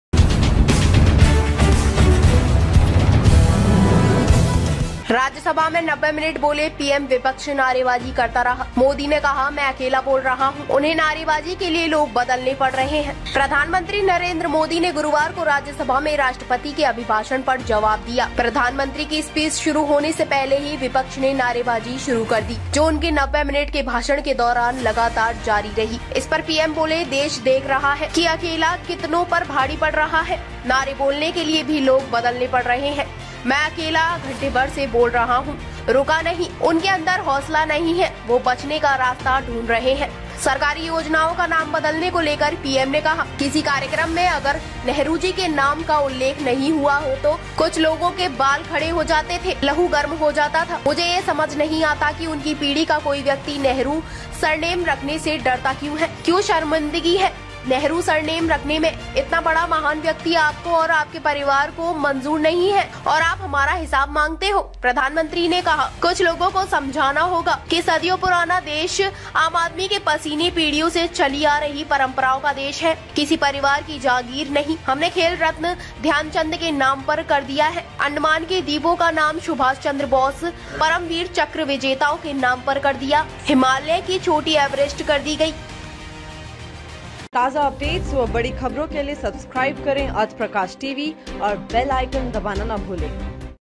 राज्यसभा में 90 मिनट बोले PM
प्रधानमंत्री नरेंद्र मोदी ने गुरुवार को राज्यसभा में राष्ट्रपति के अभिभाषण पर जवाब दिया। प्रधानमंत्री की स्पीच शुरू होने से पहले ही विपक्ष ने नारेबाजी शुरू कर दी, जो उनके 90 मिनट के भाषण के दौरान लगातार जारी रही। इस पर PM बोले- देश देख रहा है कि एक अकेला कितनों पर भारी पड़ रहा है। नारे बोलने के लिए भी लोग बदलने पड़ रहे हैं। मैं अकेला घंटेभर से बोल रहा हूं, रुका नहीं। उनके अंदर हौसला नहीं है, वो बचने का रास्ता ढूंढ रहे हैं